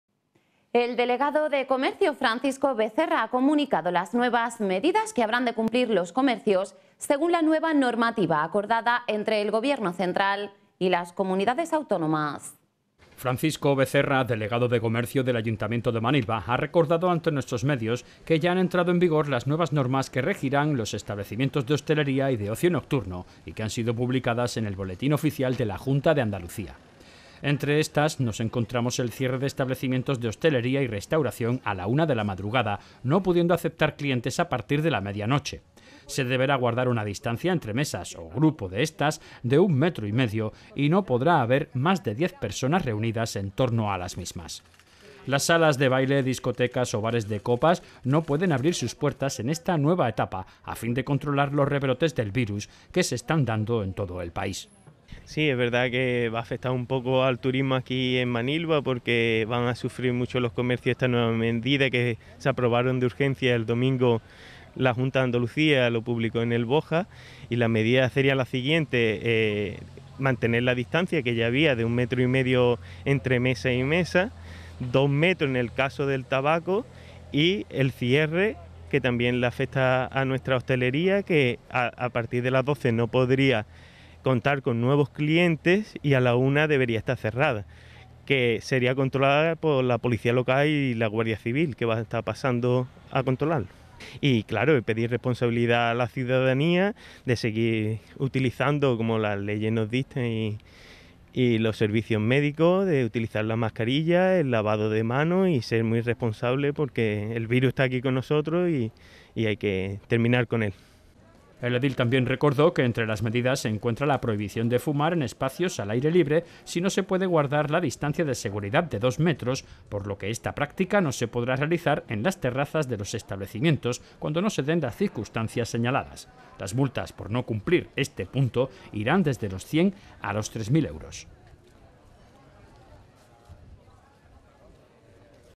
Nuevas medidas para el comercio por el Covid 19
Francisco Becerra, delegado de comercio del ayuntamiento de Manilva ha recordado ante nuestros medios,  que ya han entrado en vigor las nuevas normas que regirán a los establecimientos de hostelería y de ocio nocturno y que han sido publicadas en el boletín oficial de la Junta de Andalucía.
Entre estas nos encontramos el cierre de establecimientos de hostelería y restauración a la una de la madrugada, no pudiendo aceptar clientes a partir de la medianoche. Se deberá guardar una distancia entre mesas o grupo de estas de un metro y medio y no podrá haber más de diez personas reunidas en torno a las mismas.
Las salas de baile, discotecas o bares de copas no pueden abrir sus puertas en esta nueva etapa a fin de controlar los rebrotes del virus que se están dando en todo el país.
El edil también recordó que entre las medidas se encuentra la prohibición de fumar en espacios al aire libre si no se puede guardar la distancia de seguridad de dos metros, por lo que esta práctica no se podrá realizar en las terrazas de los establecimientos cuando no se den las circunstancias señaladas. Las multas por no cumplir este punto irán desde los 100 a las 3000 €.